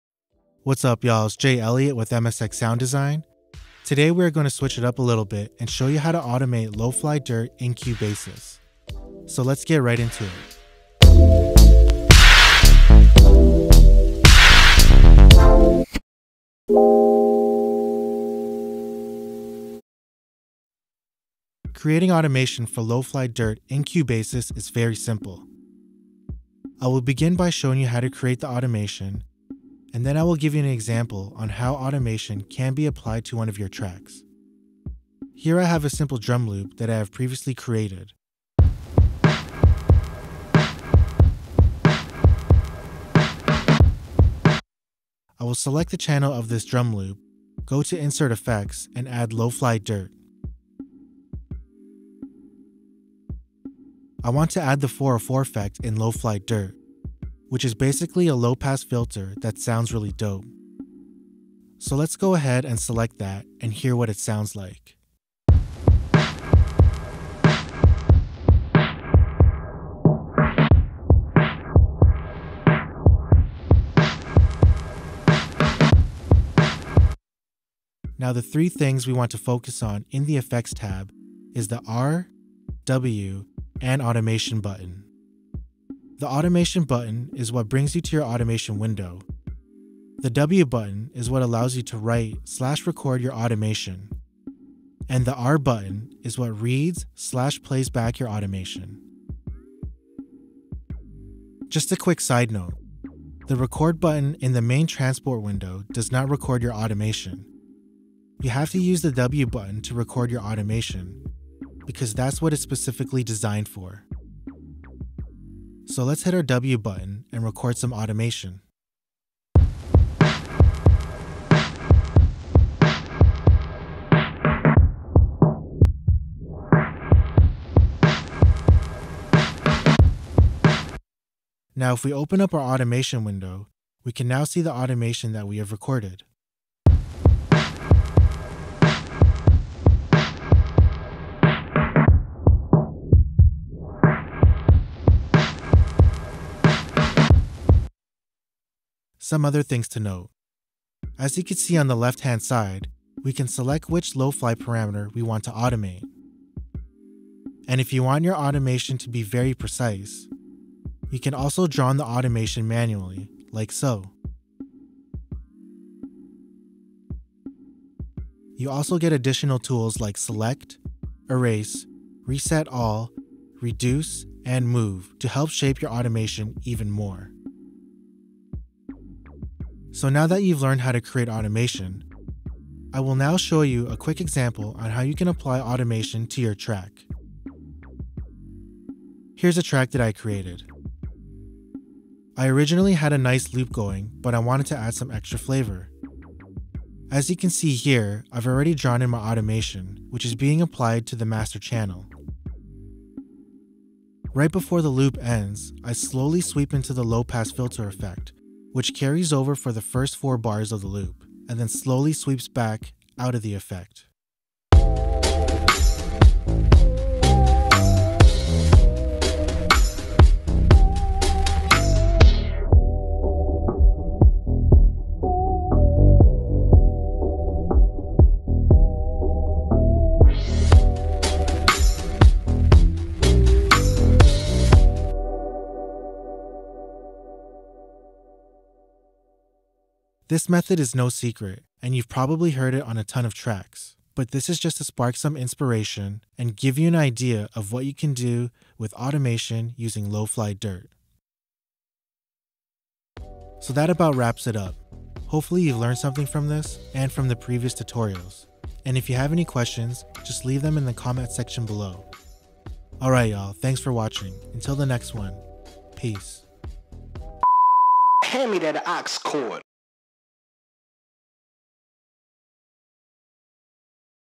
Automation with LoFly Dirt in Cubasis for iOS Beatmaking
We're back with another installment of our MSXII iOS tutorials with the fam Jay Ellyiot. Today we take a look at using automation to create movement/different sections within Cubasis. If you're not familiar, Cubasis, in our opinion, can be one of the most complete "canvas" style DAWs on iOS. Really stable, has tons of features, and works great. If you search around, tons of people are doing full tracks + recording vocals within the program!

Check the video out, let us know if you have any questions below in the comment section. As always, stay with us...much more on deck!